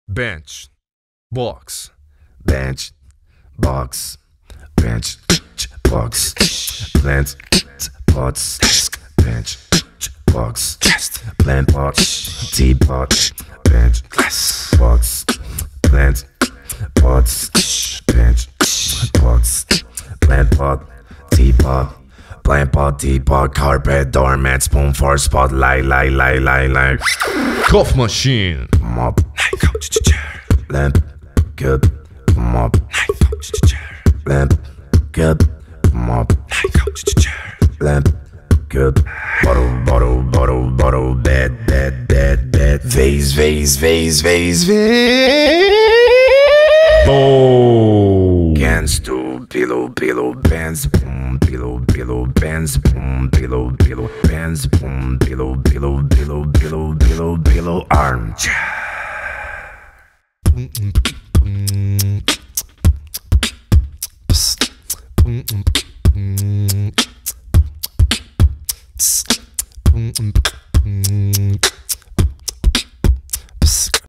ETNA | THE BEATBOX CATALOGUE
Etna is one of the biggest home and decor brands in Brazil. The brand is known for its huge catalogue and for its accessible prices. We couldn’t show their products in a common, boring way. That's why we called Caio Beatbox to present as many offers as possible in just one minute. No musical instruments were used, just his skills as a professional beatboxer. A catalogue was never so full of rhythm!